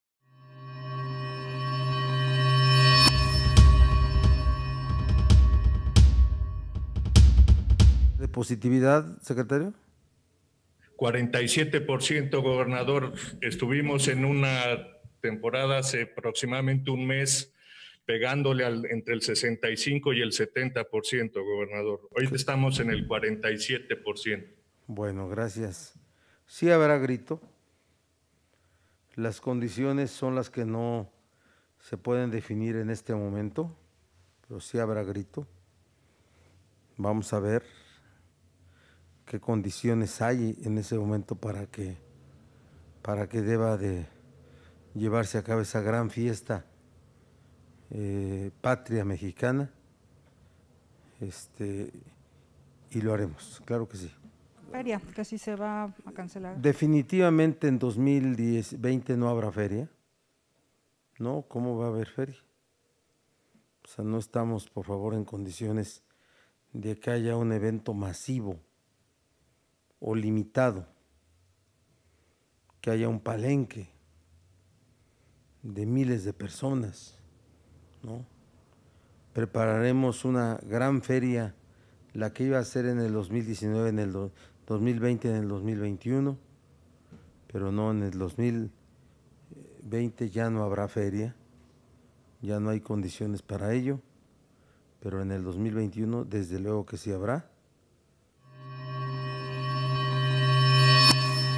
Cancelan Feria de Puebla 2020; si habrá Grito de Independencia: Barbosa
El gobernador Miguel Barbosa anunció que la Feria de Puebla queda cancelada para este 2020 ante la contingencia por COVID; Sí habrá grito este próximo 15 de septiembre.